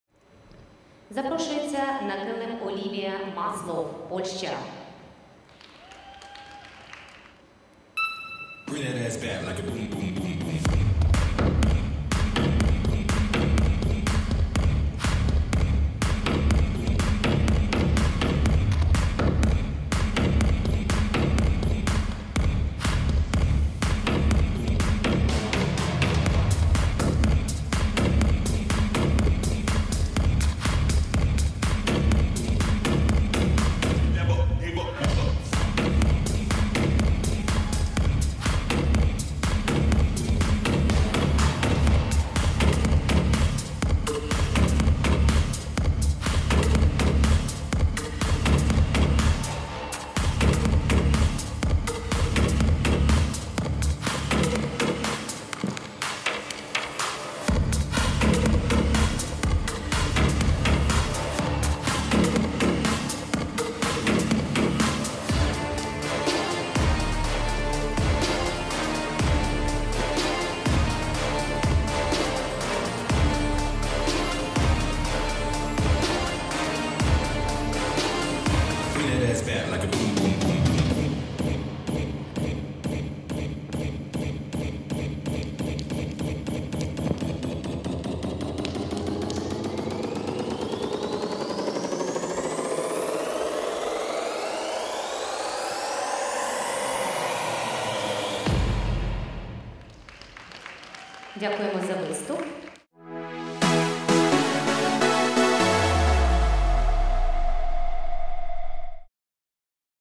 MASLOV Olivia 2009 clubs
25th International Rhythmic Gymnastics Tournament “Deriugina Cup 2020”, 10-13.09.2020, Sportyvna square 1, Kyiv 01001, Ukraine